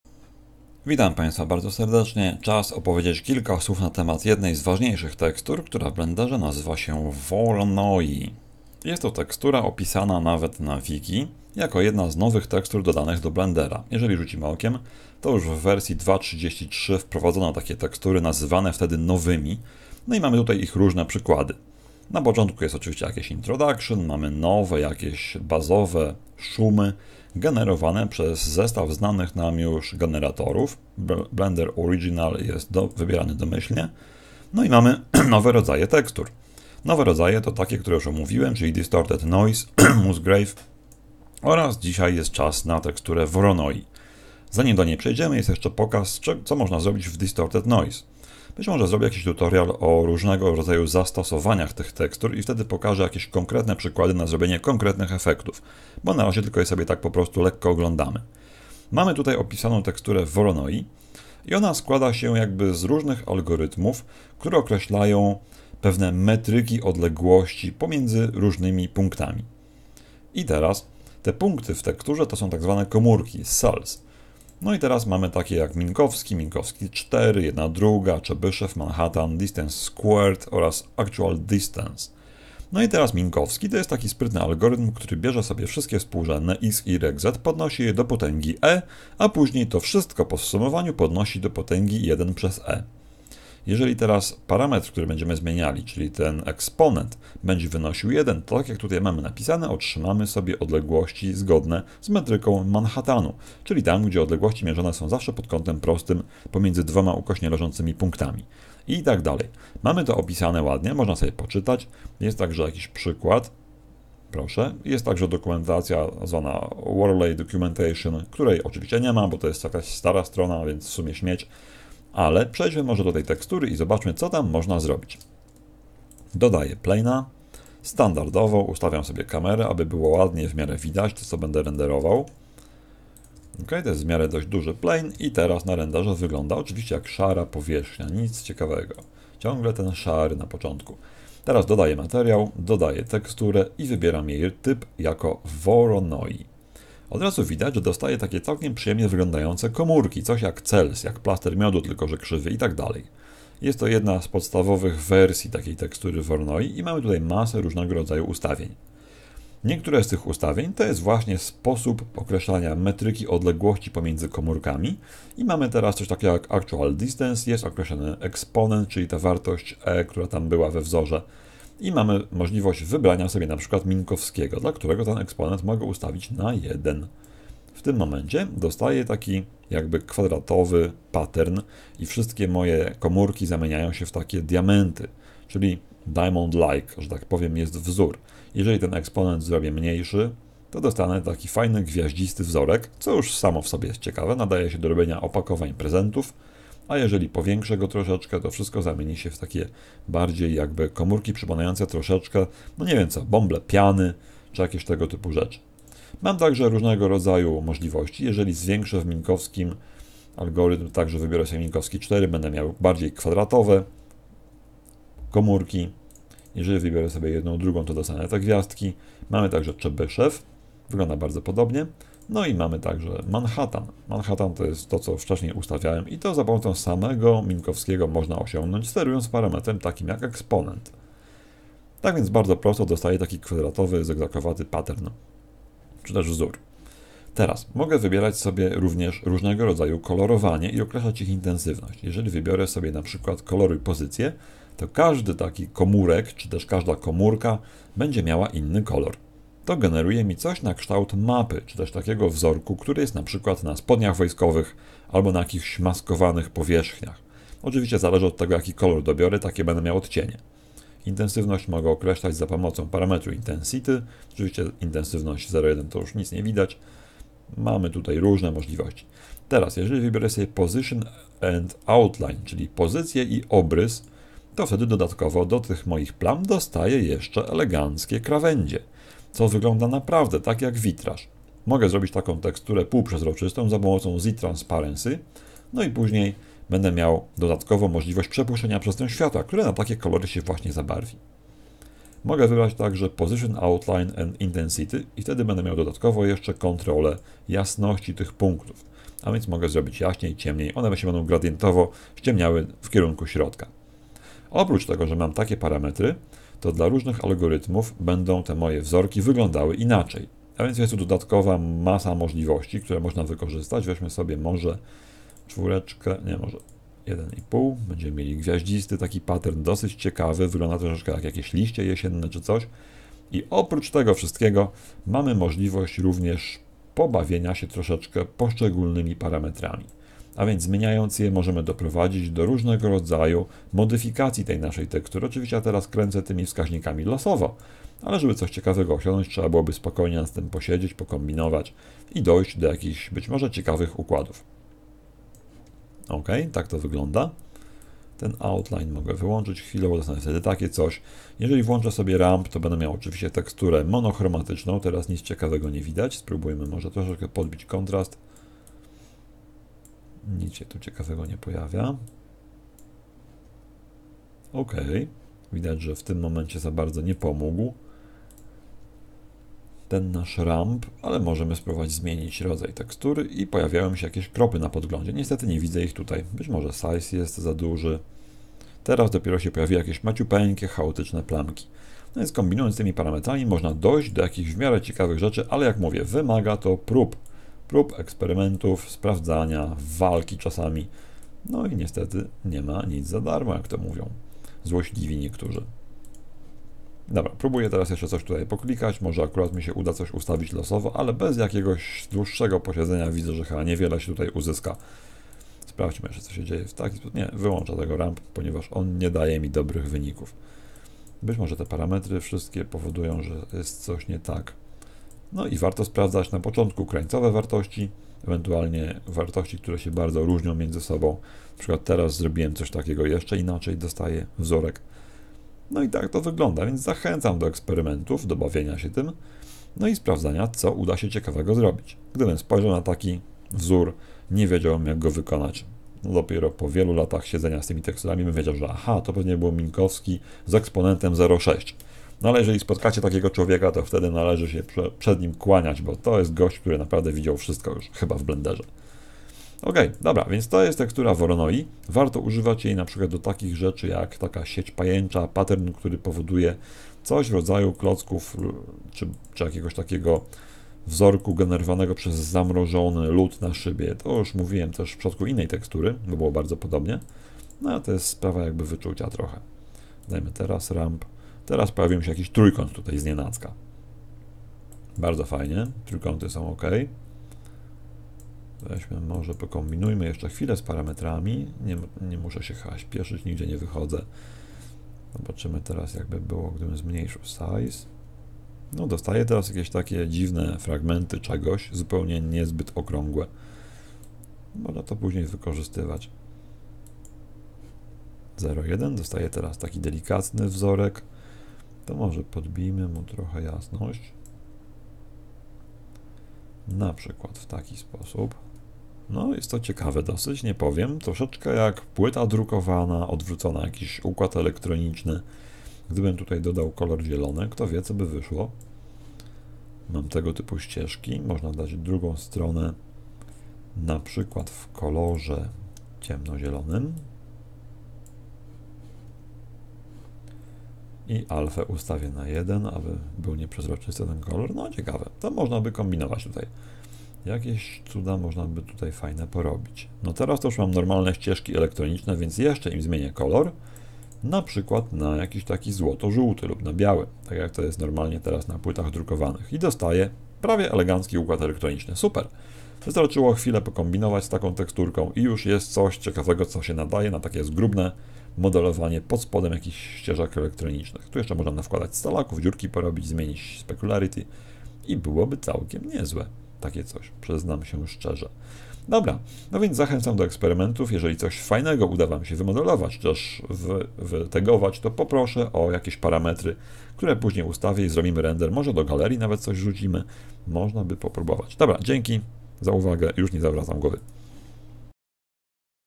Polski Kurs Blendera: Tekstury: voronoi
Poradnik wideo przedstawia jedną z ciekawszych tekstur, czyli voronoi - generowana z komórek na podstawie różnych algorytmów. Pozwala utworzyć gwiazdki lub kwadraty, lub spękania podobne go okruchów lub skorupy żółwia. Nadaje się dobrze do tworzenia pseudonaturalnych wzorów, a w połączeniu z innymi teksturami pozwala uzyskać bardzo ciekawe efekty.